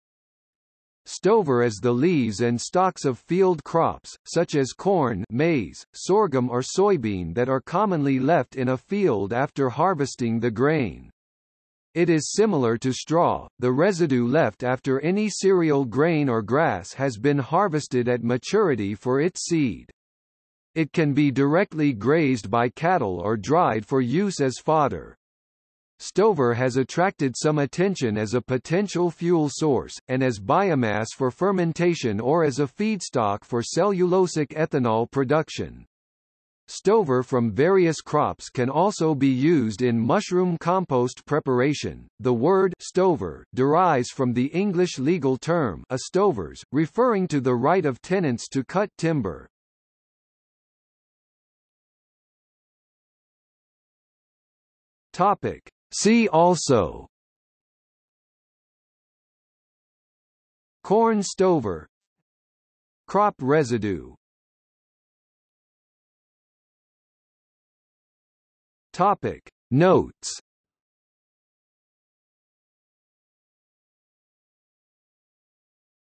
Stover | Wikipedia audio article
00:00:51 See also
00:01:05 Notes

- Socrates

SUMMARY
Stover is the leaves and stalks of field crops, such as corn (maize), sorghum or soybean that are commonly left in a field after harvesting the grain. It is similar to straw, the residue left after any cereal grain or grass has been harvested at maturity for its seed. It can be directly grazed by cattle or dried for use as fodder.   Stover has attracted some attention as a potential fuel source, and as biomass for fermentation or as a feedstock for cellulosic ethanol production.   Stover from various crops can also be used in mushroom compost preparation.The word 'stover' derives from the English legal term 'estovers', referring to the right of tenants to cut timber.